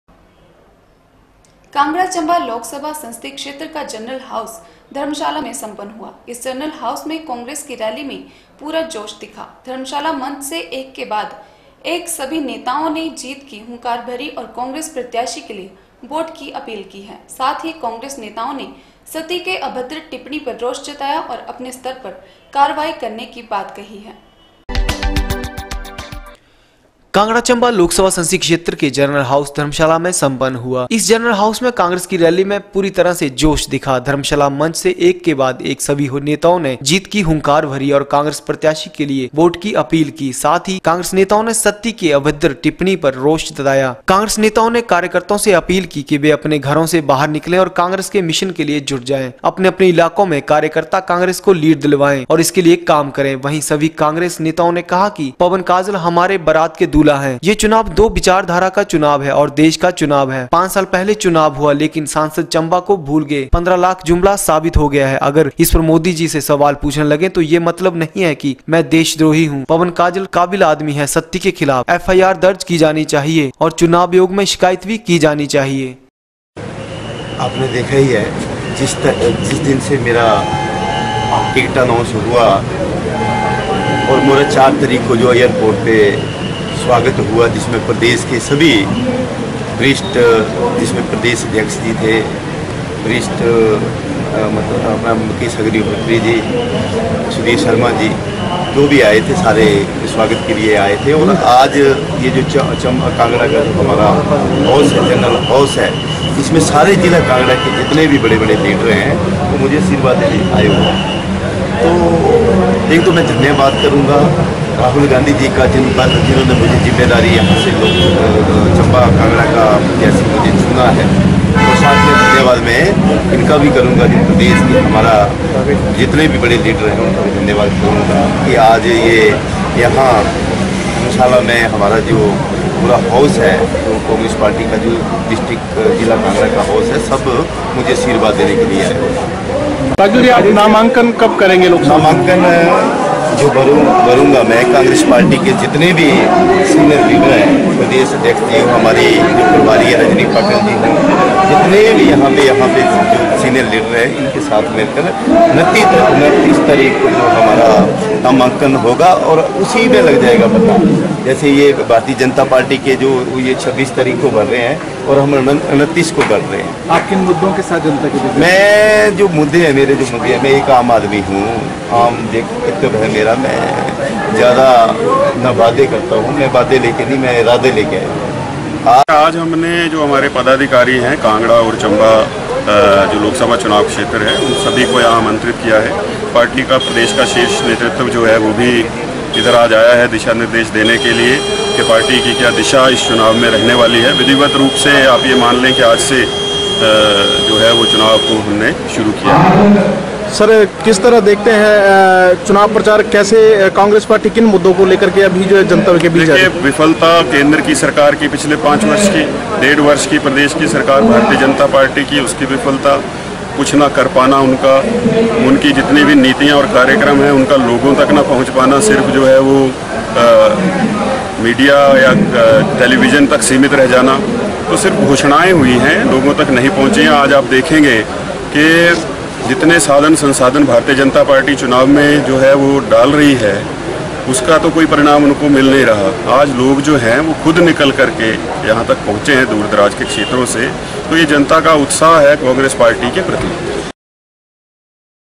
5. The enthusiasm shown in the Congress rally at dharamshala
कांगड़ा चंबा लोकसभा संसदीय क्षेत्र का जनरल हाउस धर्मशाला में सम्पन्न हुआ इस जनरल हाउस में कांग्रेस की रैली में पूरा जोश दिखा। धर्मशाला मंच से एक के बाद एक सभी नेताओं ने जीत की हुंकार भरी